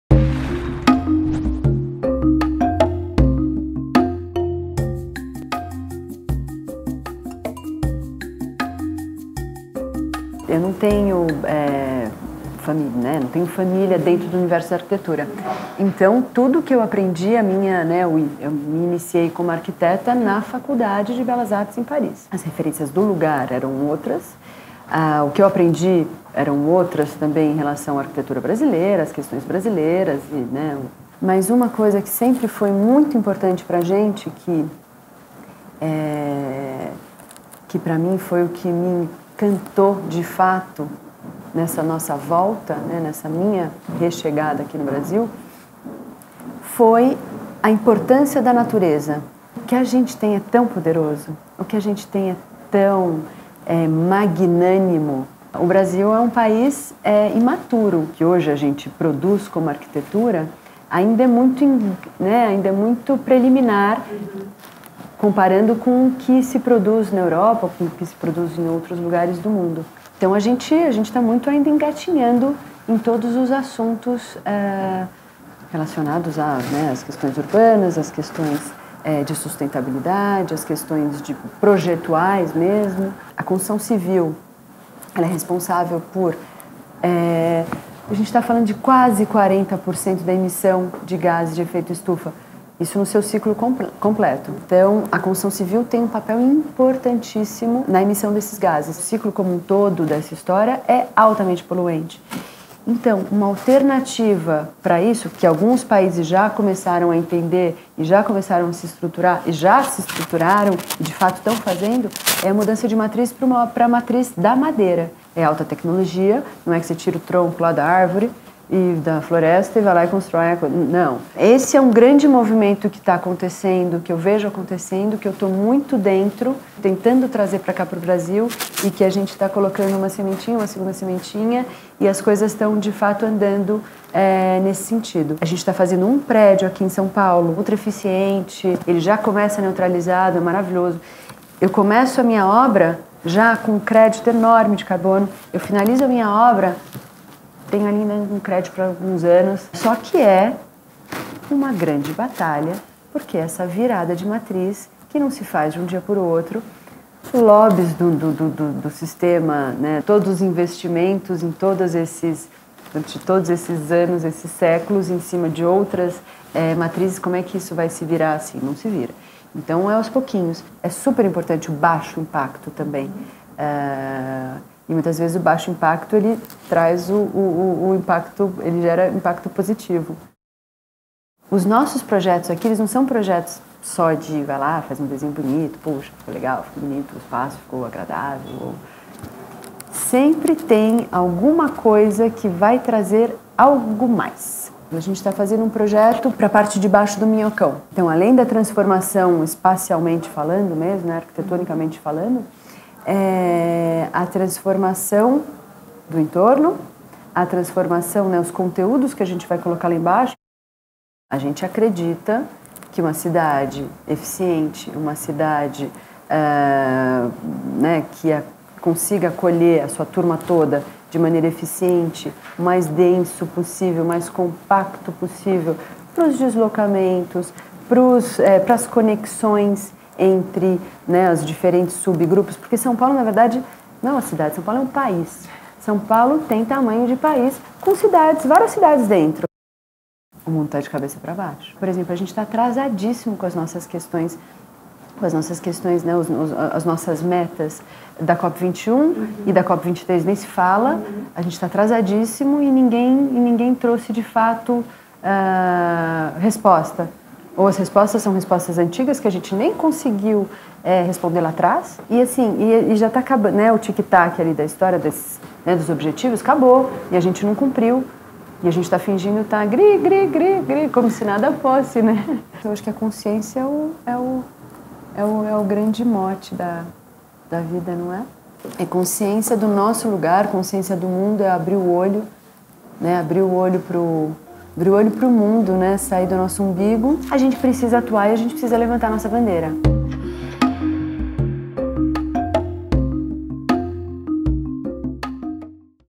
Agentes Transformadores - Carol Bueno
Carol Bueno Arquiteta e sócia do escritório de arquitetura Triptyque, valoriza os projetos que possuam responsabilidade social e ambiental. Há 18 anos ela montou o Triptyque com franceses que conheceu na Escola de Belas Artes de Paris. Premiado dentro e fora do Brasil, sempre buscou criar projetos que dialogassem com o seu entorno. Seus projetos já se tornaram referência, como o edifício de escritórios da Rua Harmonia, onde a água da chuva e do solo é tratada e reutilizada pelo prédio, vencedor do Zumtobel Group Award por sua sustentabilidade. E o edifício da Rua Fidalga em que cada apartamento é diferente do outro que foi selecionado para a Bienal de Veneza de 2009.